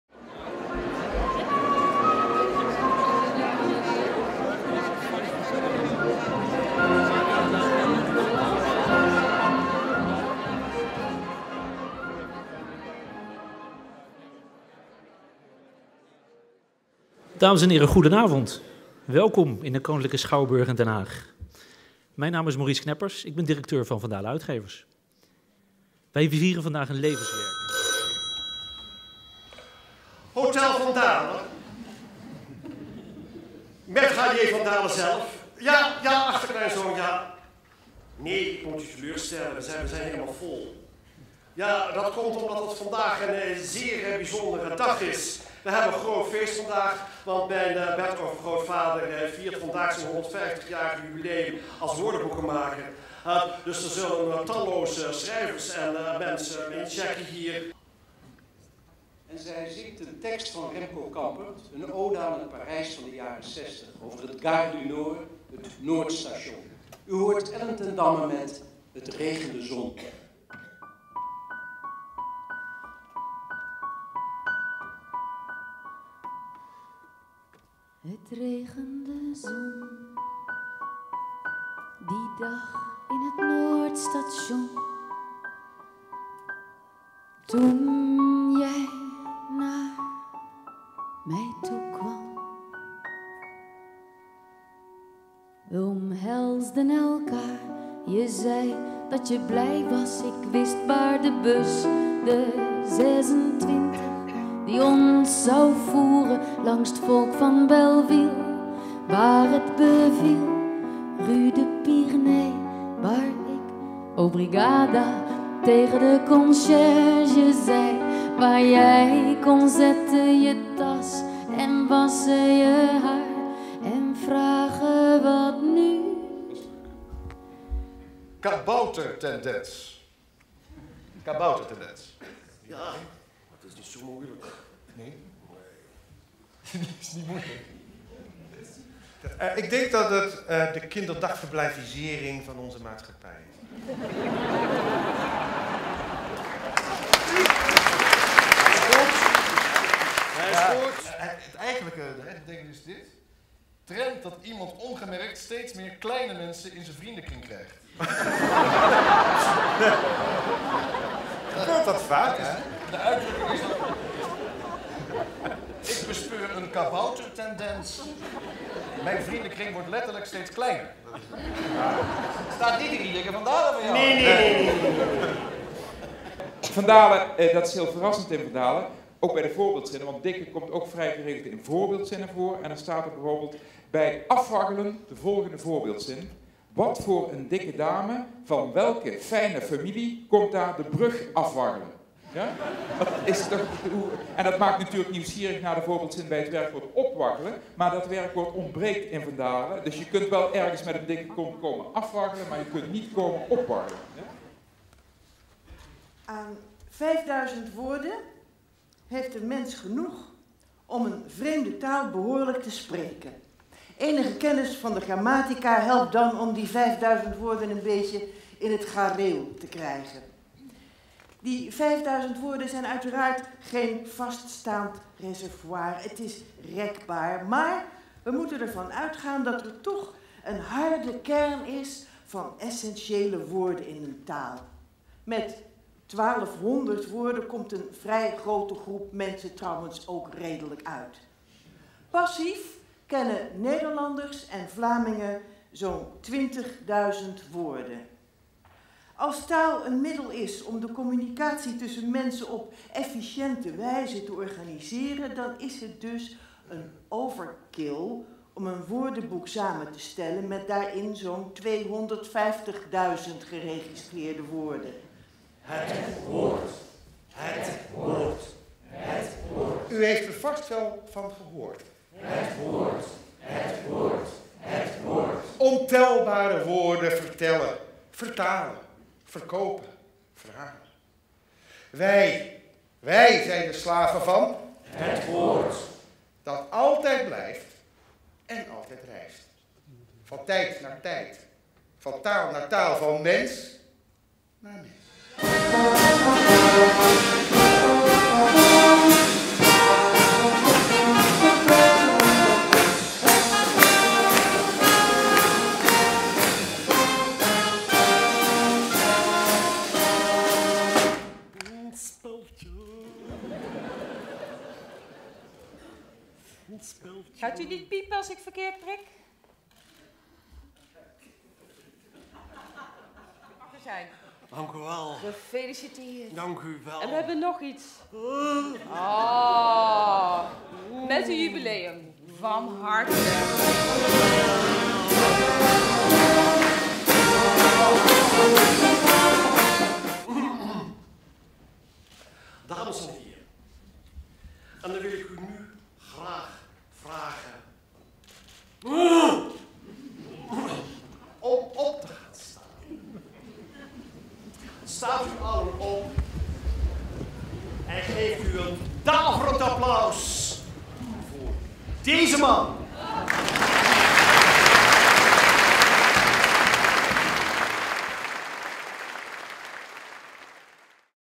Hotel Van Dale - compilatie
Het 150-jarig jubileum van Van Dale werd feestelijk gevierd op donderdag 28 augustus 2014 in de Koninklijke Schouwburg in Den Haag. De schouwburg was voor deze speciale gelegenheid omgedoopt tot ‘Hotel Van Dale, het hotel waar woorden wonen’.
Arjan Ederveen ontving als ‘bellboy van dienst’, maar vooral als betachterkleinkind van Johan Hendrik van Dale, in Hotel Van Dale gasten die incheckten voor het jubileumfeest. Betalen deden ze niet met geld, maar met woorden en taal. Gedichten, odes, woordgrappen, muziek en zang, uitzending op de hotelradio, taalspelletjes en anekdotes vielen de jarige Dikke Van Dale en het publiek ten deel.
Naast Arjan Ederveen traden deze gasten op in Hotel Van Dale: Ellen ten Damme, Menno Wigman, Nelleke Noordervliet, Vincent Bijlo, Tjitske Jansen, The Kik, Wim Daniëls, Menno Wigman, Zazí, Hans Dorrestijn, Frits Spits, Ellen Deckwitz, Ronald Snijders, Eppo van Nispen, Bart Chabot, Geert Joris, Justus van Oel, burgemeester Annemiek Jetten van Sluis en de Koninklijke Muzieksociëteit ‘Aardenburgsche Fanfaren’. Zij allen gaven de 150-jarige met hun bijzondere bijdragen een prachtig cadeau.